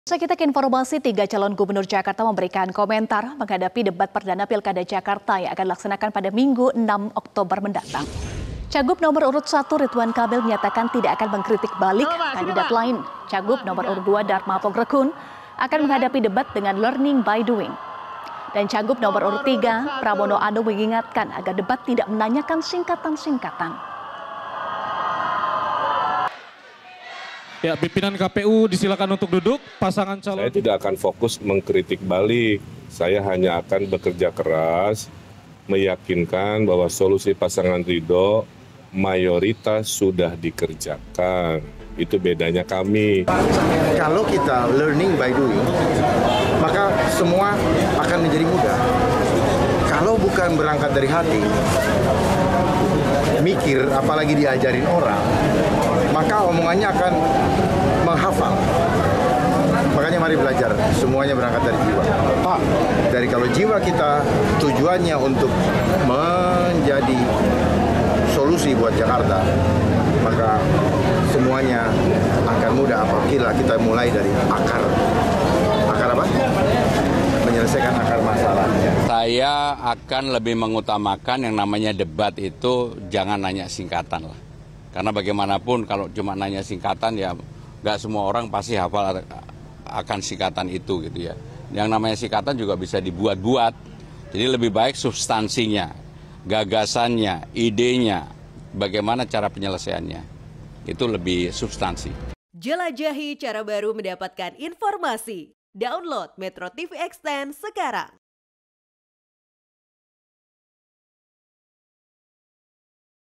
3 Calon Gubernur Jakarta Berkomentar Jelang Debat - [BERITA PILKADA]
MetroTV, Tiga calon gubernur Jakarta memberi komentar menghadapi debat perdana pilkada jakarta yang akan dilaksanakan minggu 6 oktober nanti.